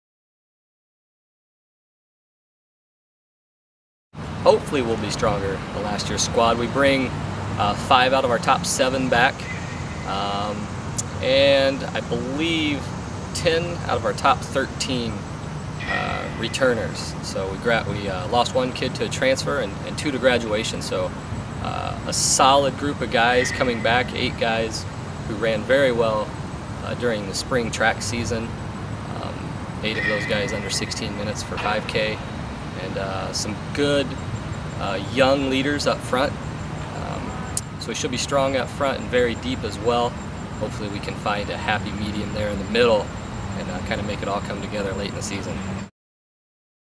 Cross Country Preview
Wabash College head cross country coach Roger Busch talks about the expectations for his 2009 squad.